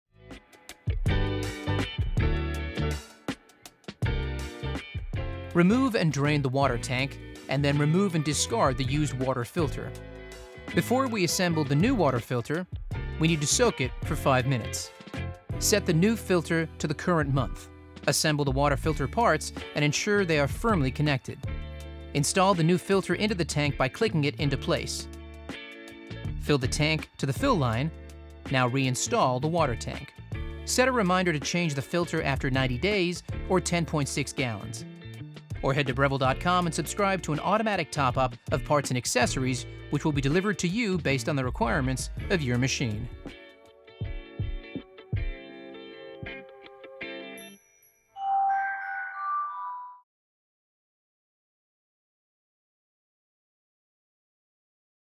The Barista Touch™ | How to replace the water filter on your espresso machine | Breville USA
Why it is important to maintain a clean water filter.

Replacing the water filter on your coffee machine eliminates the bitter taste of your coffee. This is why we have created a step-by-step guide on how you can change the filter to achieve rich, full-flavored coffee every time.

Directions:
1. Remove and drain the water tank.
2. Then, remove and discard the used water filter.
3. Set the new filter to the current month and assemble the water parts. Ensure they are firmly connected.
4. Install the new filter into the tank by clicking it into place.
5. Fill the tank to the MAX line and soak for 5 minutes.
6. Reinstall the water tank in to place and remember to the replace the filter after 90 days or after 10.6 gallons.